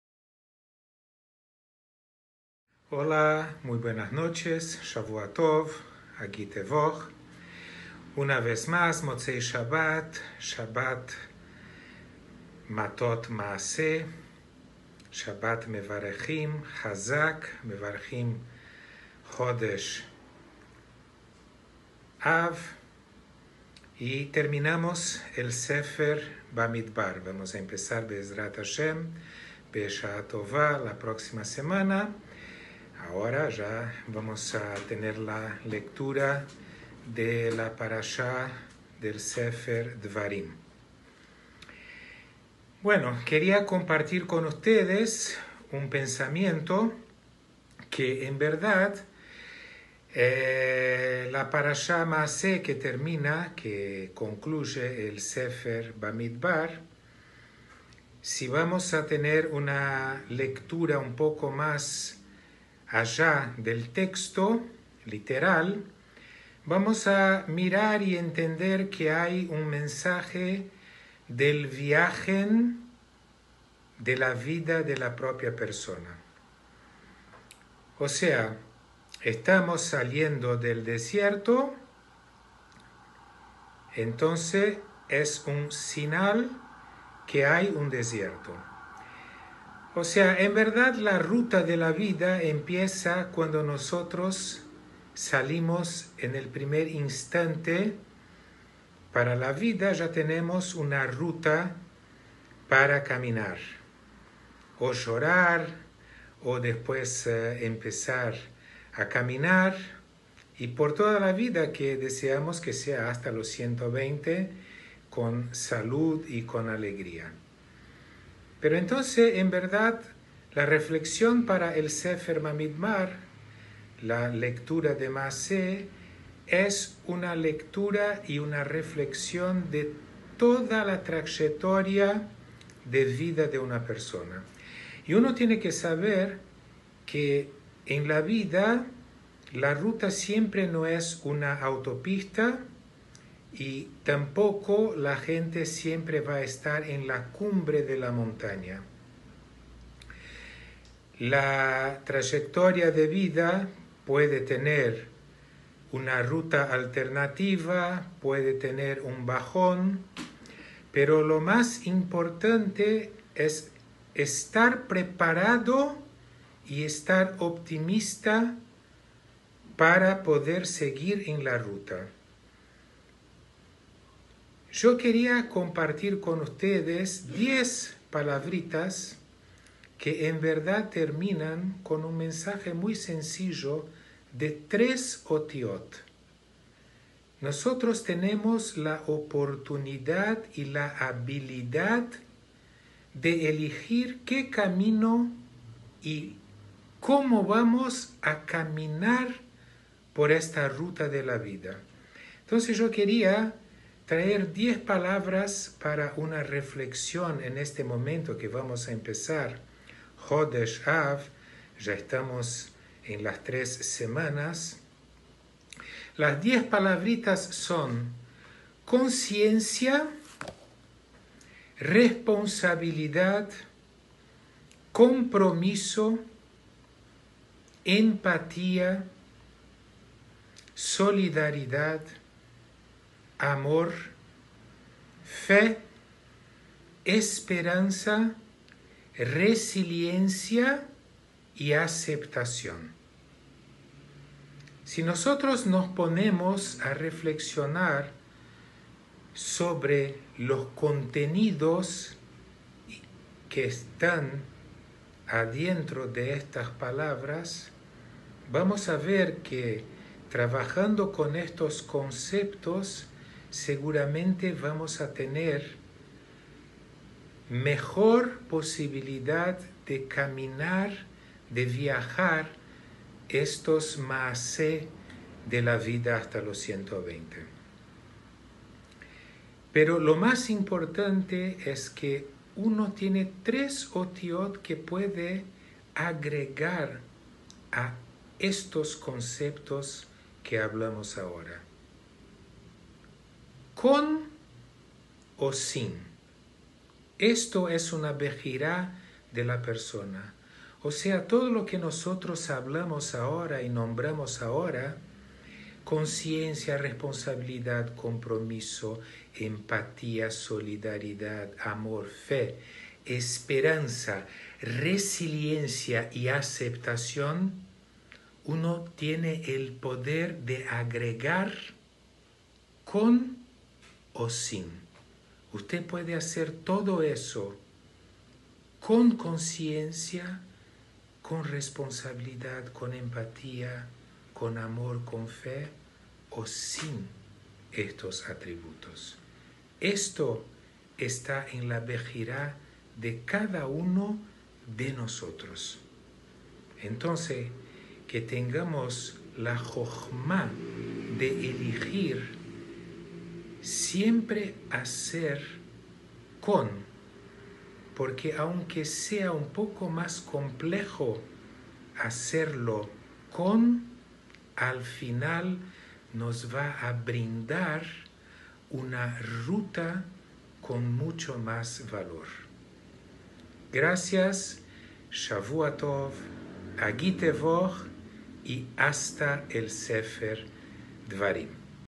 Gute Vokh - Dr. Nathan Gontow - 19 Julio
Cada Motzaé Shabbat, un mensaje desde Brasil.
Reflexiones y pensamientos de Torah para comenzar la semana